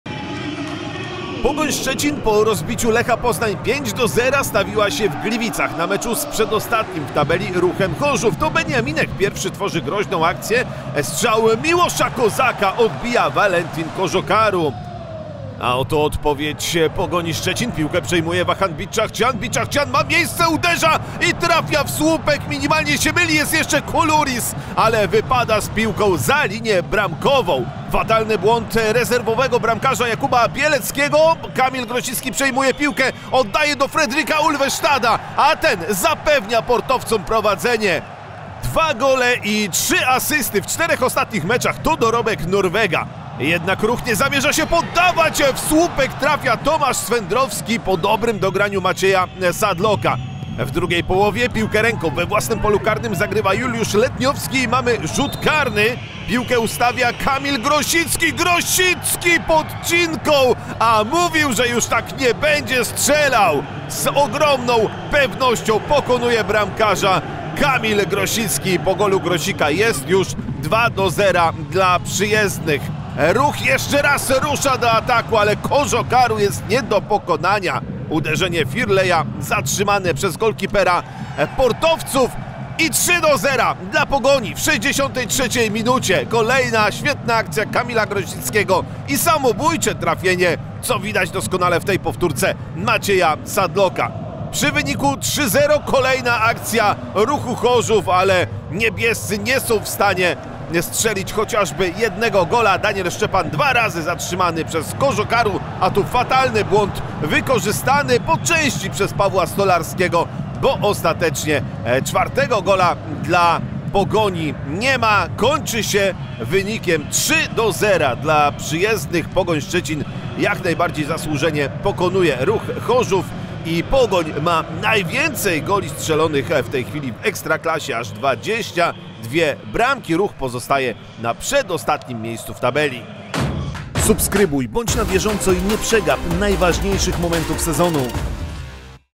RUCH - POGOŃ | SKRÓT | TRZY GOLE “PORTOWCÓW”! | Ekstraklasa 2023/24 | 11. kolejka
W 11. kolejce PKO BP Ekstraklasy Ruch Chorzów przegrał 0:3 z Pogonią Szczecin.

Ruch Chorzów - Pogoń Szczecin 0:3
0:1 Fredrik Ulvestad (28’)
0:2 Kamil Grosicki (57’ - rzut karny)
0:3 Maciej Sadlok (63’ - bramka samobójcza)

Zobacz Więcej!